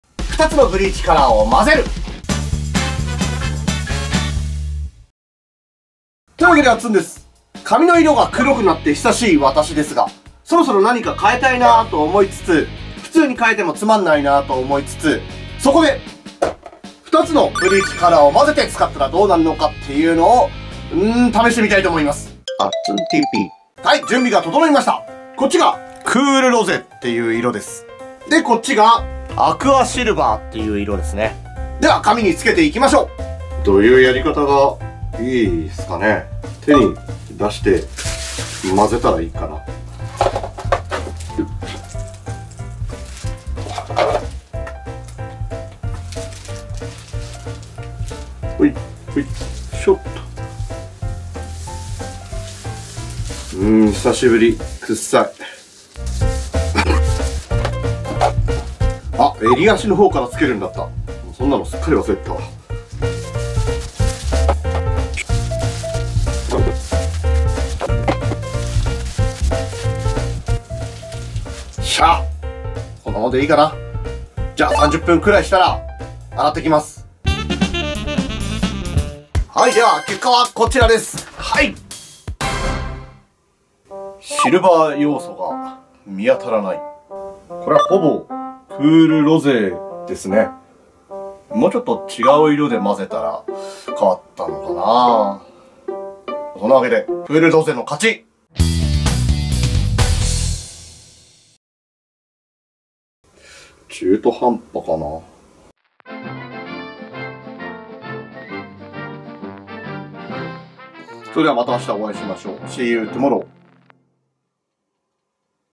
二つのブリーチカラーを混ぜたらどうなる？
ブリーチカラーを混ぜたら何色になるのか実験